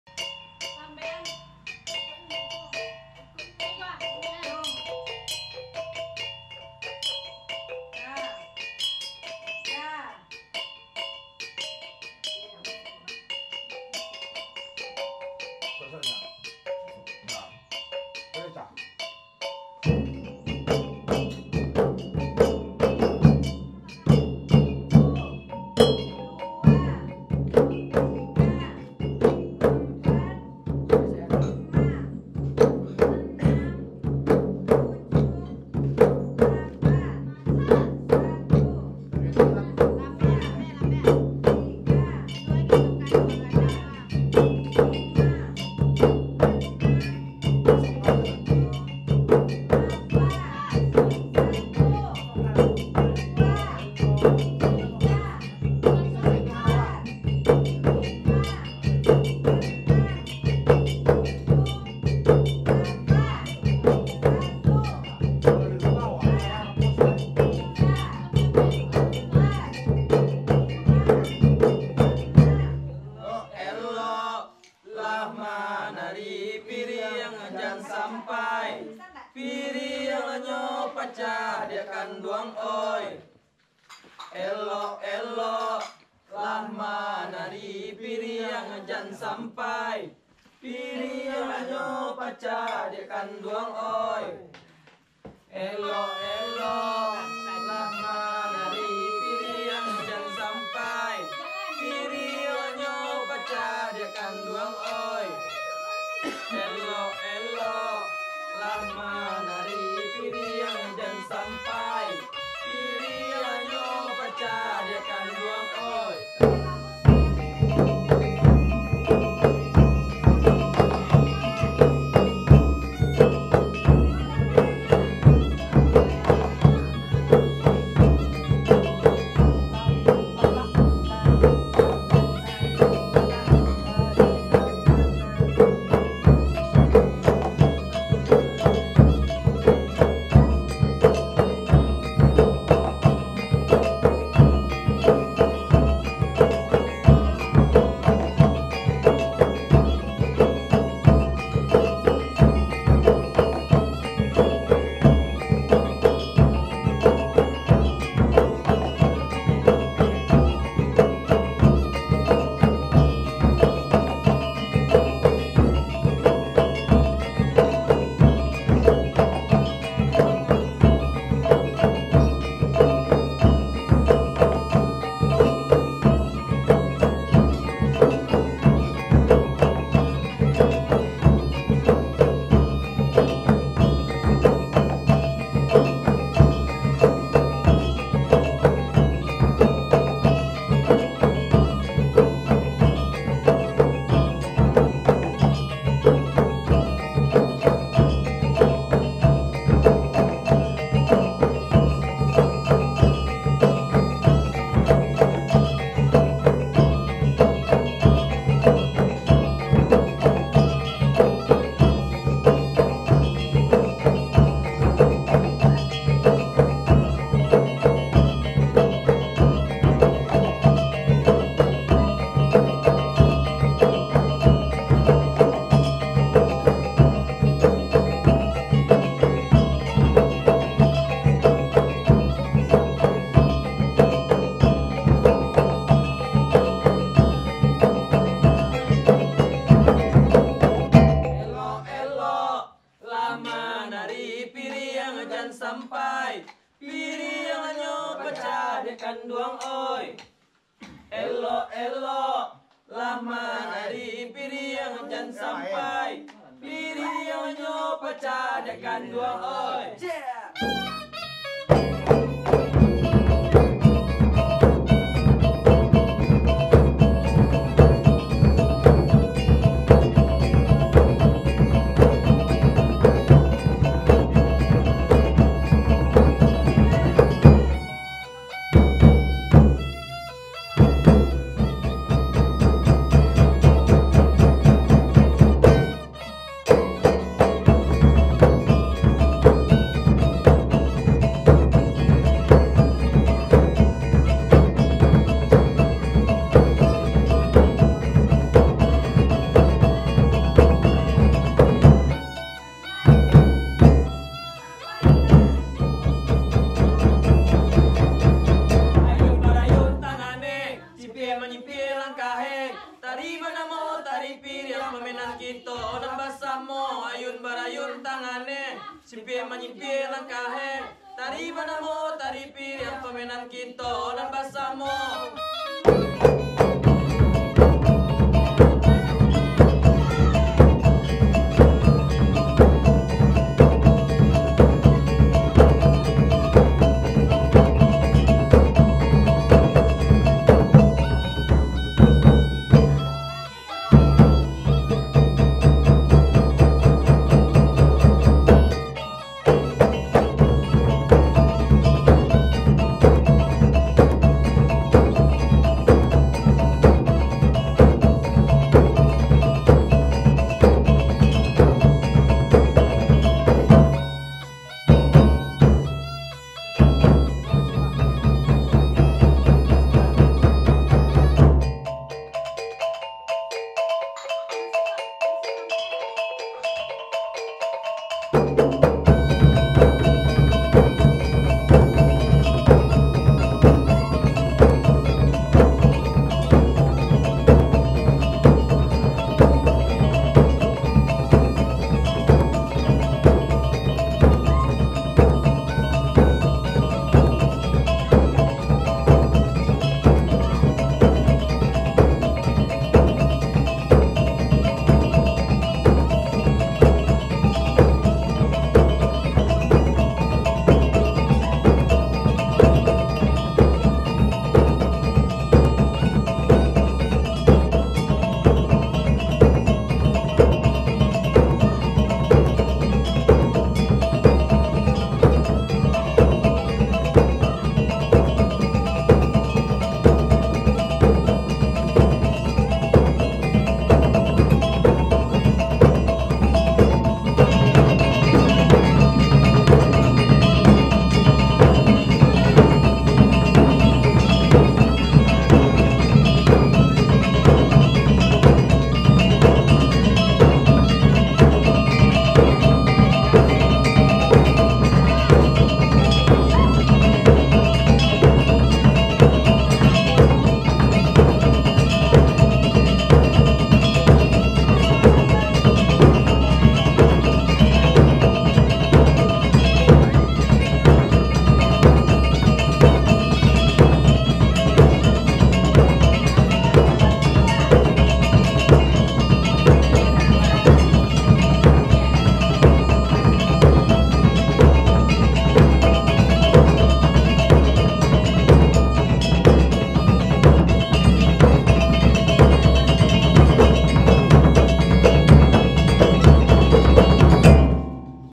Musik Tari Piring diateh karambia || Pemusik Cam || Sesi Latihan
LPPM UNP
Lembaga Penelitian dan Pengabdian kepada Masyarakat

Program Pengembangan Produk Unggulan Daerah (PPPUD)

Judul : Pelatihan dan Workshop Tari Piriang di ateh Karambia kepada Pelajar Sekolah Dasar dan Menengah di Nagari Payo Kota.

Tim PPPUD
Ketua : Irdhan Epria Darma Putra
Anggota : Wimbrayardi & Venny Rosalina

Tim Musik Tari :
Hendri Yusuf
Eka Febrio Aryandi
Ade Maulana
Zulgian Rahmadanel
Farid Ikhsan

Dokumentasi
Riki Cungak
Abak Jonai

SOLOK